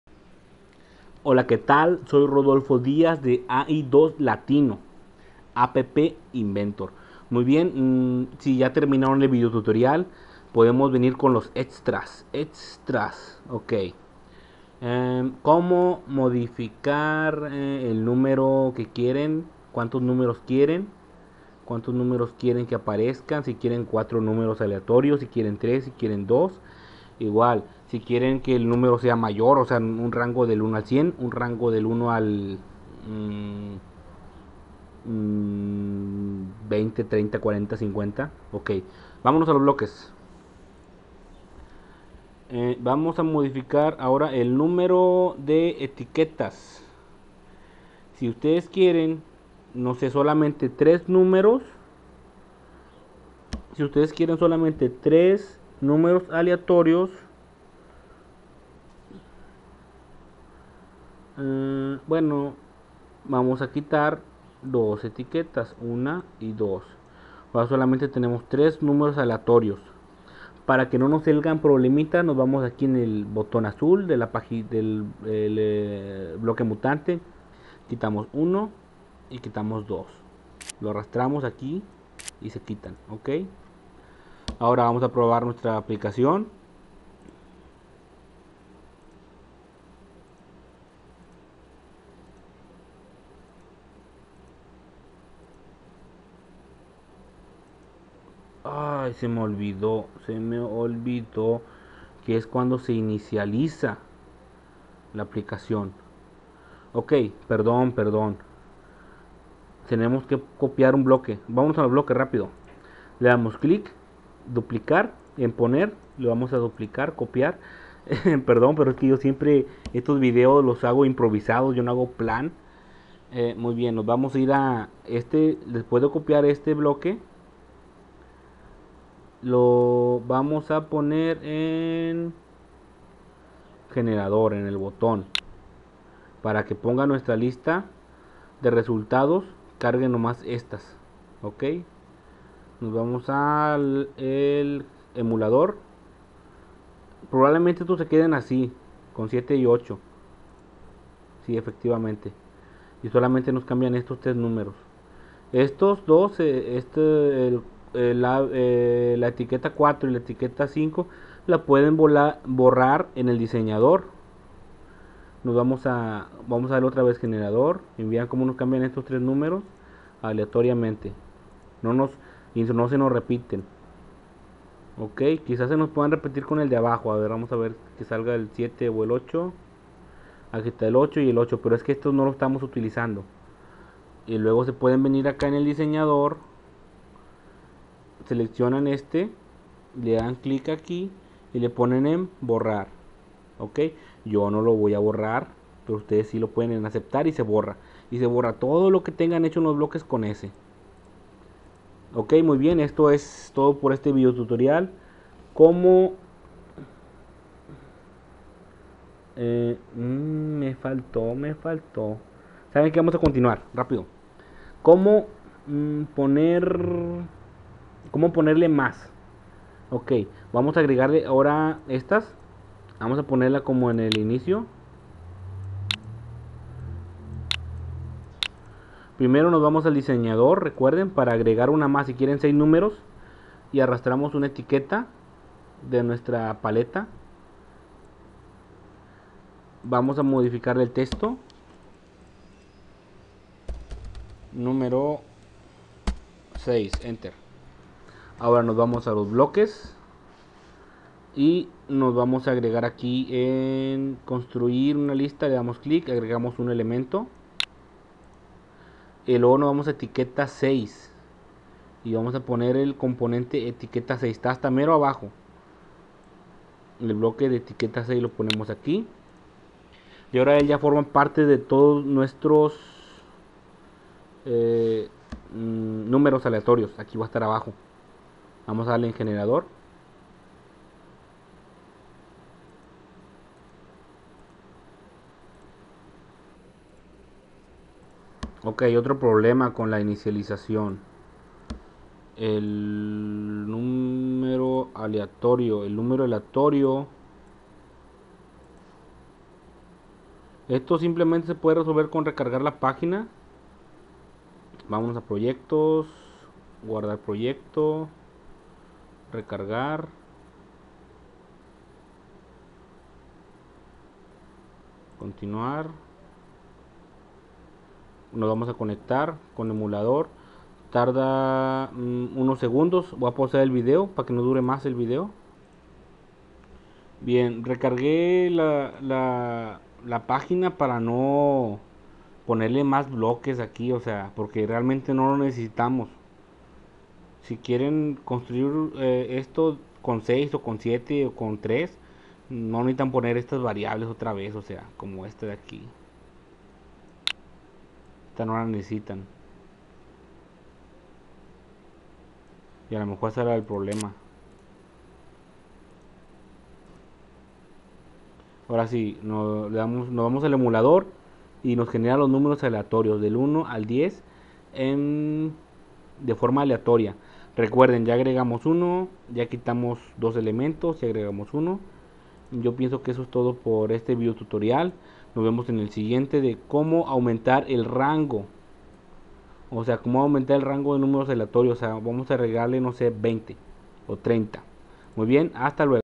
Extra quitar o poner numeros generador de numero aleatorios app inventor ai2
en este tutorial mostraremos como hacer una app que genere numeros aleatorios. disfrutenla